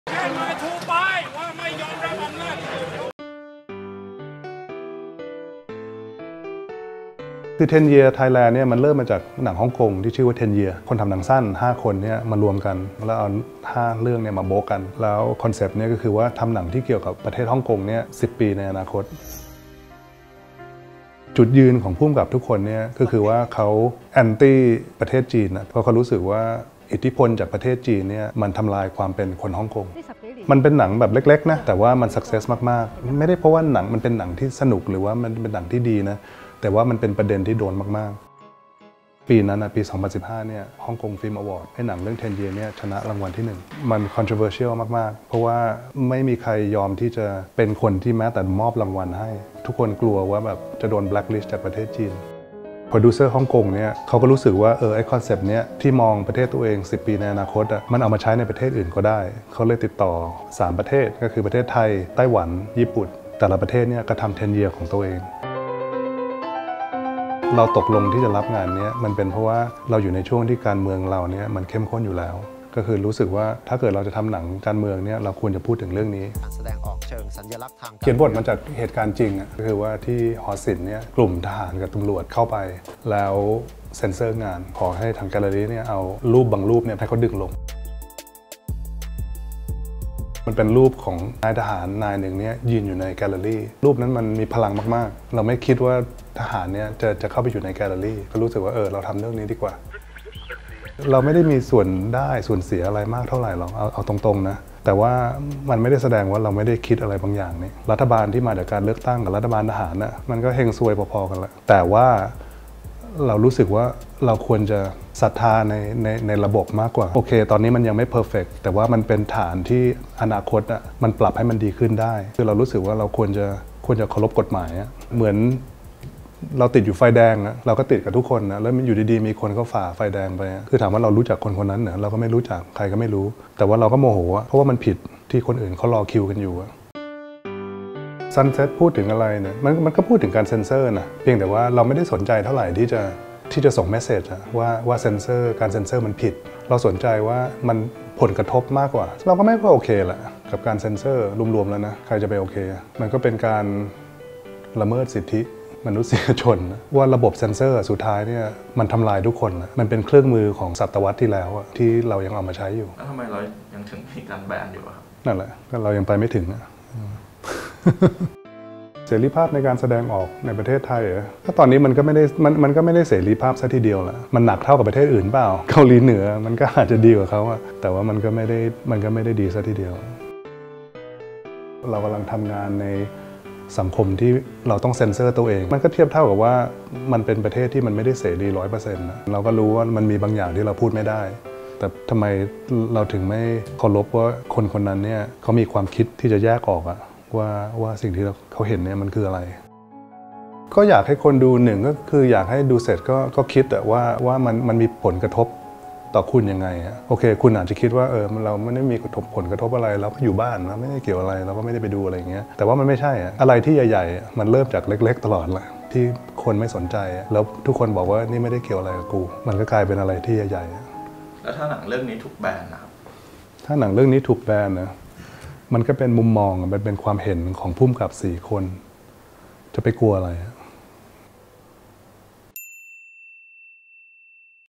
TEN YEARS THAILAND ดินแดนแห่งเสรี?
“การเซ็นเซอร์มันก็เป็นการละเมิดสิทธิมนุษยชน มันเป็นเครื่องมือของศตวรรษที่แล้วที่เรายังเอามาใช้อยู่” หนึ่งประโยคจากผู้กำกับภาพยนตร์อิสระ อย่าง ‘อาทิตย์ อัสสรัตน์’ ที่ทำให้เราต้องกลับมาฉุกคิดว่า ทำไมประเทศเรายังคงมีเหตุการณ์แบบนี้ขึ้นซ้ำแล้วซ้ำเล่า และอดตั้งคำถามกลับไปไม่ได้ว่า ทำไมการจำกัดเสรีภาพในการแสดงออกทางความคิดอ่านยังเป็นสิ่งที่สังคมเราเพิกเฉยอยู่
จากสถานการณ์การเมืองที่ไม่ปกติทำให้ ‘จุ๊ก - อาทิตย์ อัสสรัตน์’ ตอบรับโปรเจกต์การทำภาพยนตร์ TEN YEARS THAILAND และสร้างสรรค์ภาพยนตร์เรื่อง SUNSET ให้เกิดขึ้น โดยจับประเด็นร้อนในสังคมอย่างกรณีทหารสั่งปลดภาพในหอศิลป์ กรุงเทพฯ มาถ่ายทอดแง่มุมการเมืองในแบบของเขา แล้วอะไรคือความไม่ย่อท้อในการสร้างผลงาน และไม่ยอมให้อะไรหรือใครมาตีกรอบทางความคิดของผู้กำกับคนนี้ มาติดตามกัน
Line: @brandThink (มี @ ด้วยนะครับ)